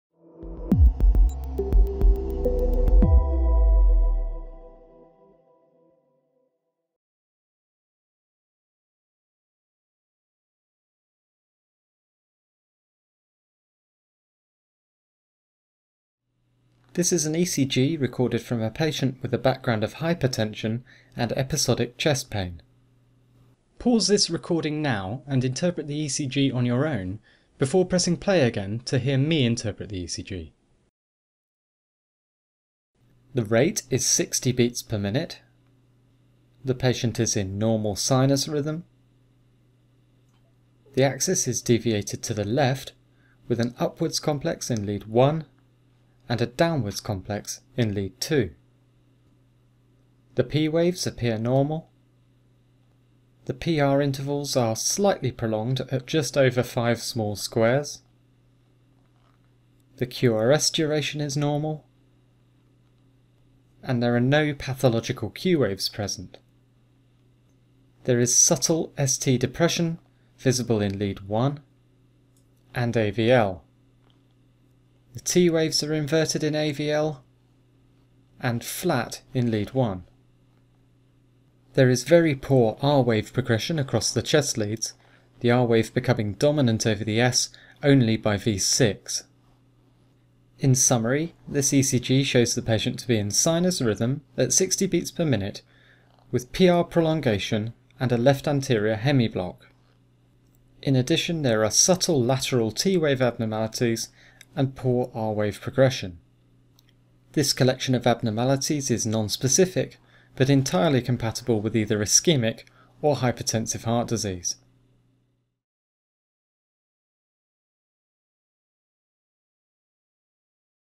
ECG Case 10 - Episodic Chest Pain
63 year old woman sees her GP with episodic chest pain. She has a background of hypertension. This ECG was recorded in the absence of symptoms.